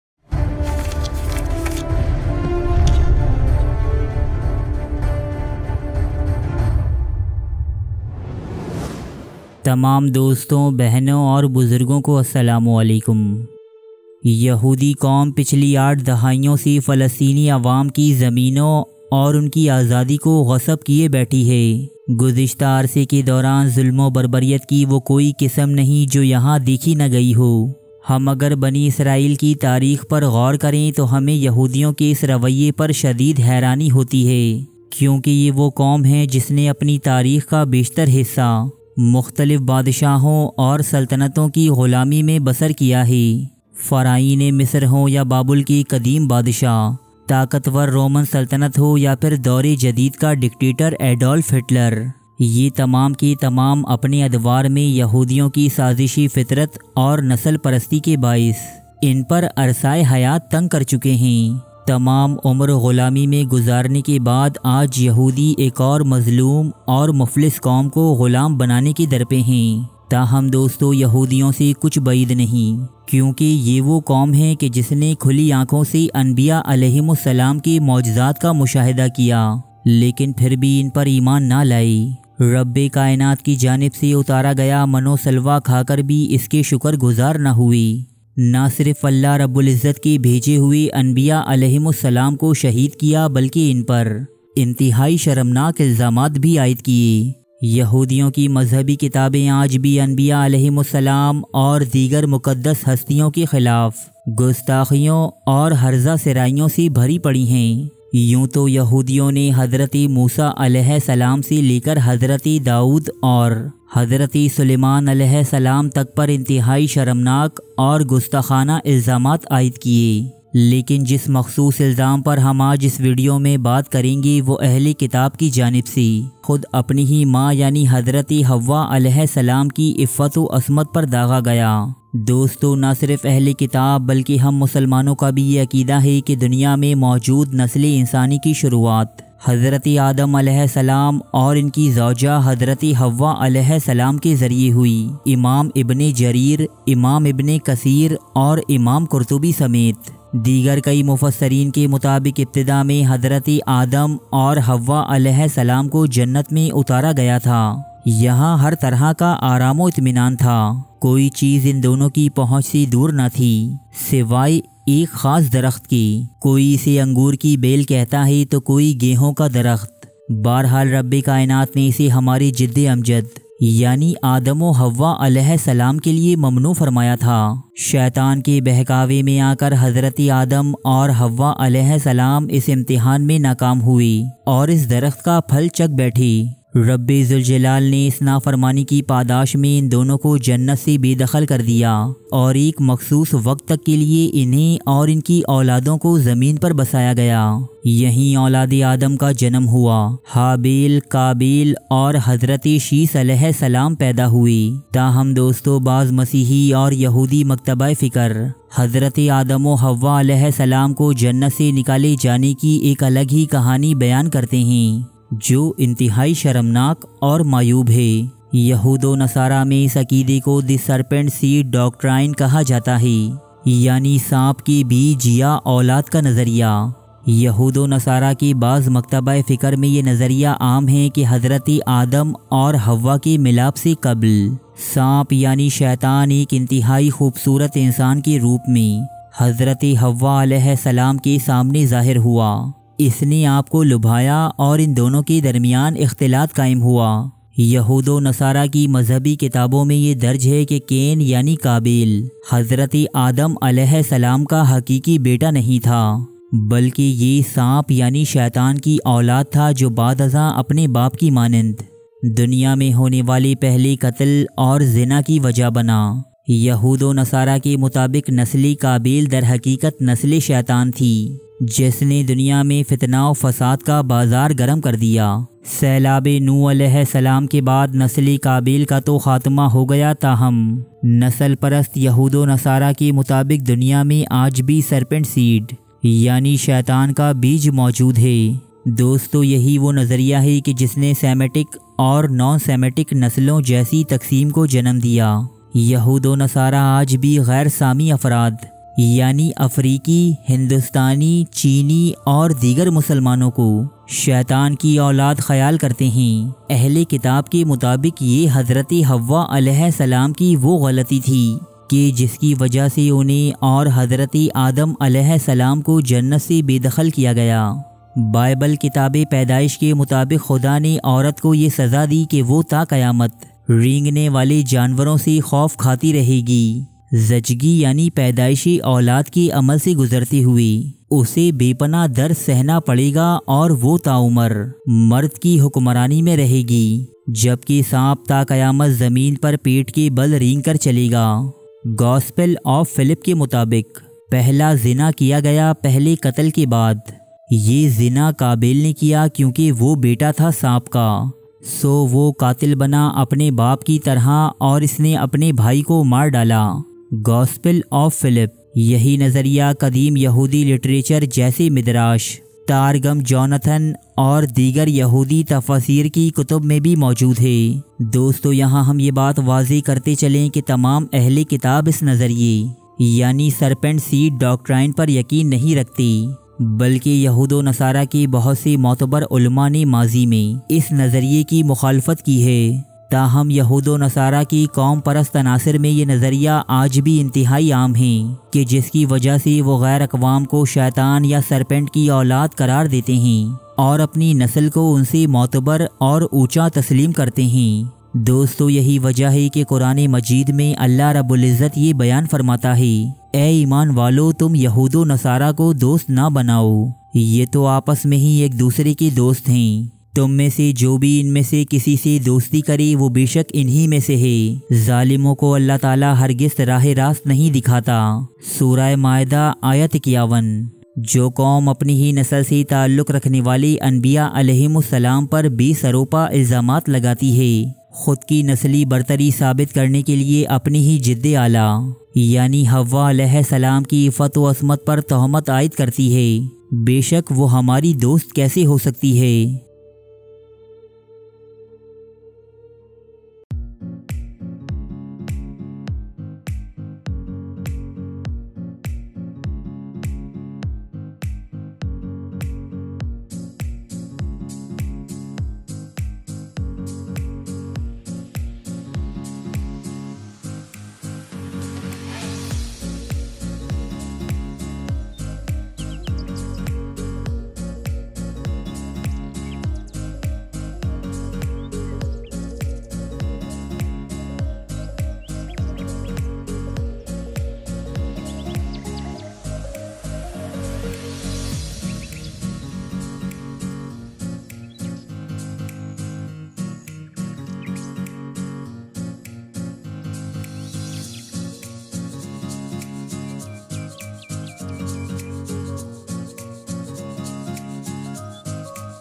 What is Serpent Seed Explained in Hindi & Urdu!
Asslam o Alekum Dosto...! Humari aaj ki video Ehl e Kitab ke ek intehaye sharamnak nazariye par sawal uthati hai, jesa ke hum jante hain ke yahoodi duniya ki wo qom hain ke jinhon ne anbiya A.S ki taleemat ko na Sirf jhutlaya balke unhain shaheed karne tak se peeche na hate... yahoodiyon ki kitaben anbiya A.S ki gustakhiyon se bhari pari hain... lekin humari aaj ki video ek makhsoos ilzam par hai jo wo Hazrat Amma Hawwa par ayed karte hain... serpent seed doctrine kiya hai aur ye konsa nazariya hai ye tamam baten aap jaan saken ge humari aaj ki video main.